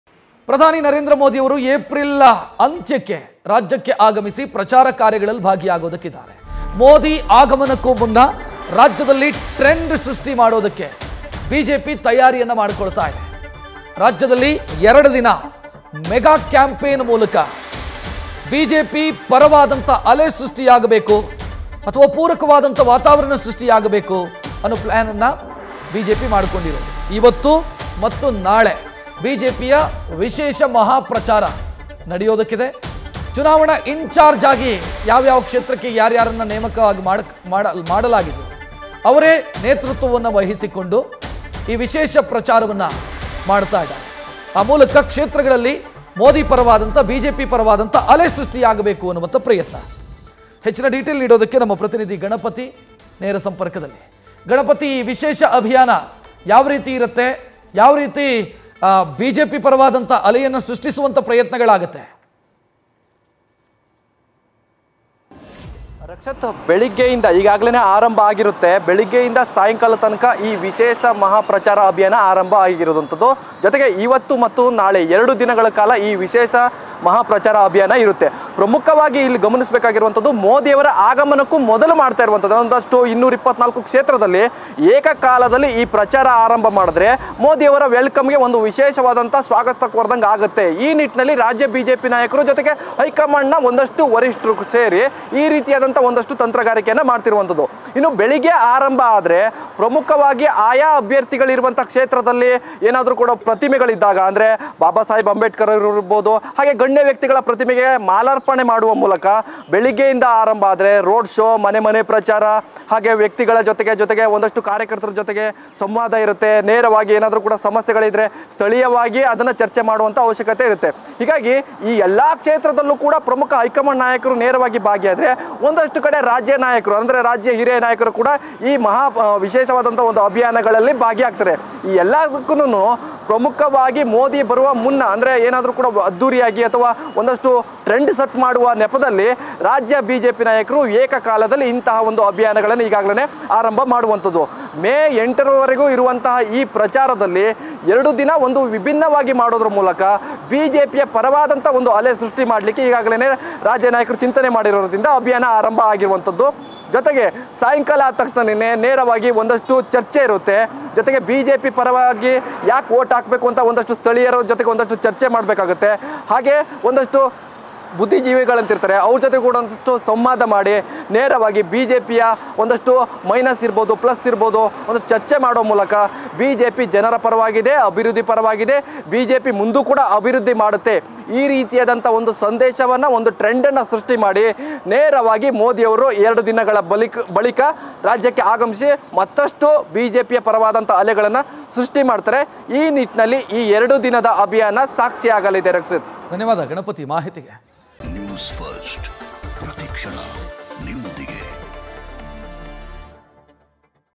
PM Narendra Modi ಆಗಮನಕ್ಕೂ ಮುನ್ನ ಟ್ರೆಂಡ್​ ಸೃಷ್ಟಿಸಲು ತಯಾರಿ | Amit Shah | Election 2023 | NewsFirst
PM Narendra Modi ಆಗಮನಕ್ಕೂ ಮುನ್ನ ಟ್ರೆಂಡ್​ ಸೃಷ್ಟಿಸಲು ತಯಾರಿ | Amit Shah | Election 2023 | @newsfirstkannada